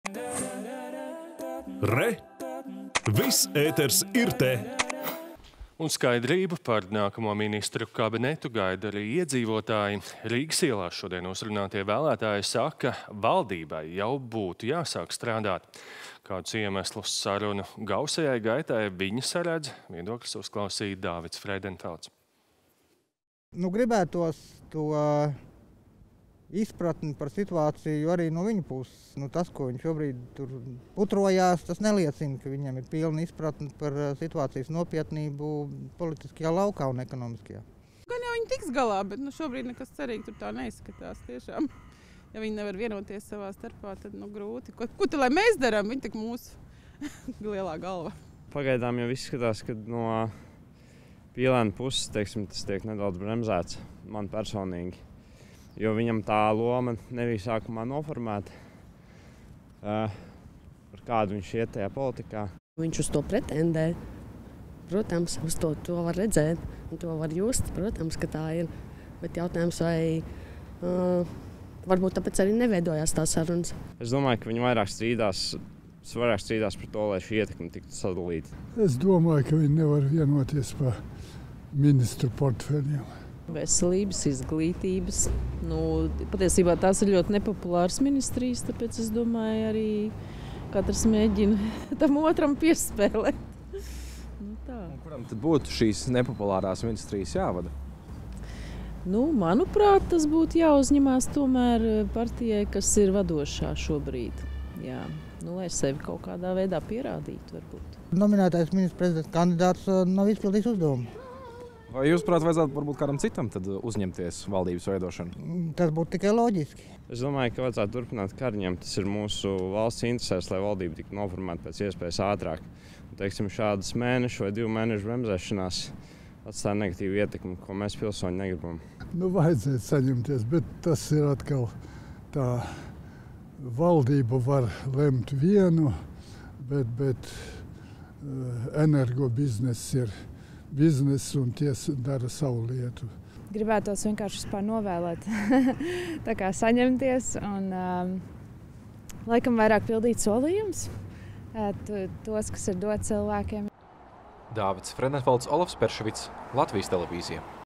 Iedzīvotāji: Koalīcija nespēj sadalīt ministru krēslus
Skaidrību par nākamo Ministru kabinetu gaida arī iedzīvotāji. Rīgas ielās šodien uzrunātie vēlētāji saka – valdībai jau būtu jāsāk strādāt. Kādus iemeslus sarunu gausajai gaitai viņi saredz, viedokļus uzklausīja Dāvids Freidenfelds.